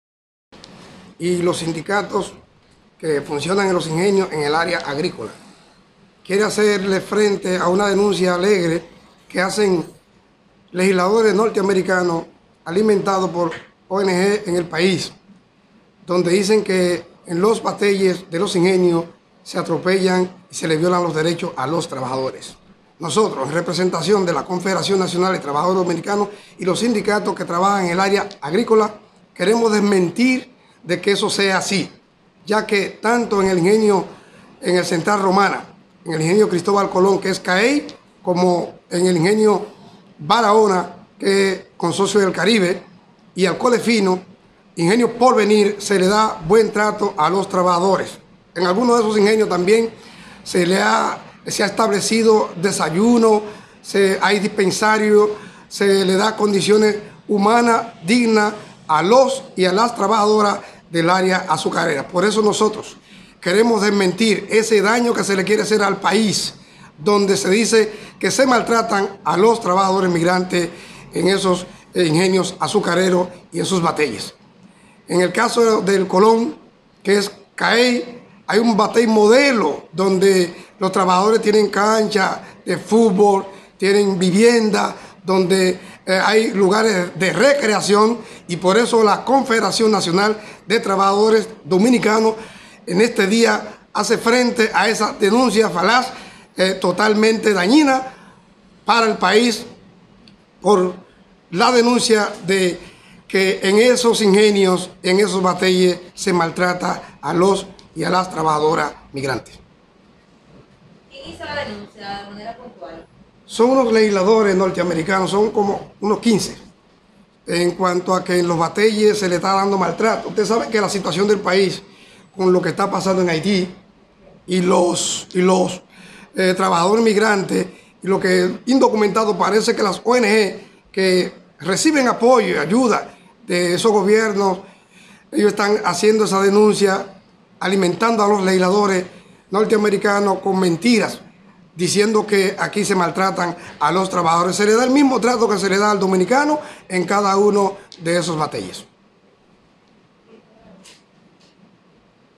Trabajadores desmienten que se violen derechos a los migrantes en la clase obrera
La Confederación de Trabajadores Dominicanos (CNTD) desmintieron este martes que en los bateyes de los ingenios se les violan los derechos a los migrantes de la clase obrera.